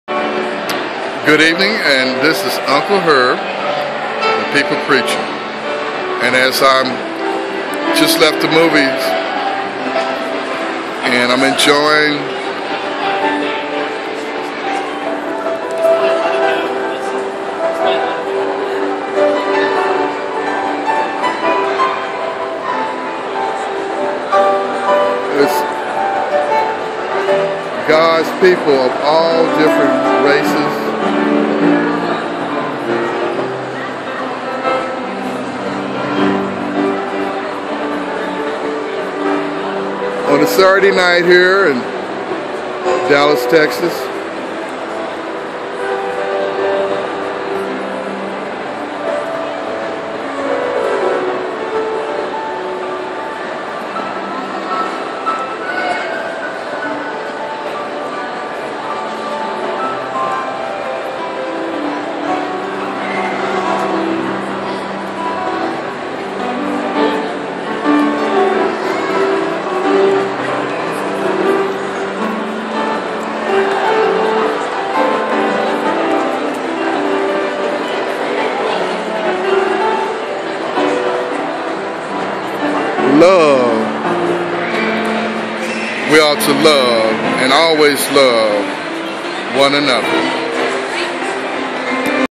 We Are To Love One Another!
John 13:34  (34) "A new command I give you: Love one another.  As I have loved you, so you must love one another."

Isaiah 40:5 (5) And the glory of the Lord will be revealed, and all people will see it together.  For the mouth of the Lord has spoken.

Revelation 7:9 (9) After this I looked, and there before me was a great multitude that no one could count, from every nation, tribe, people and language, standing before the throne and before the Lamb. They were wearing white robes and were holding palm branches in their hands.

 أنا أحبك كثيرا - Arabic; P̄hm rạk khuṇ māk - Thai;
あなたを本当に愛しています-Japanese;
Ninakupenda sana - Swahili; te quiero mucho - Spanish;
YA tak tebya lyublyu - Russian; Volim te puno - Bosnia;
tôi yêu bạn rất nhiều - Vietnamese;
Mujhē tumasē bahuta pyāra hai - Hindi; Wǒ nàme ài nǐ - Chinese;
je t'aime tellement - French; ich liebe dich so sehr - German;
אוהב אותך - Hebrew; se agapó̱ tóso polý - Greek;
dangsin-eul neomu salang haeyo - Korean;
jag älskar dig så mycket - Swedish;
aku sangat mencintaimu - Indonesian;
Я так тебе люблю - Ukrainian; I love you so much - English!
Uncle Herb
The people preacher!
Uncle Herb
The people preacher!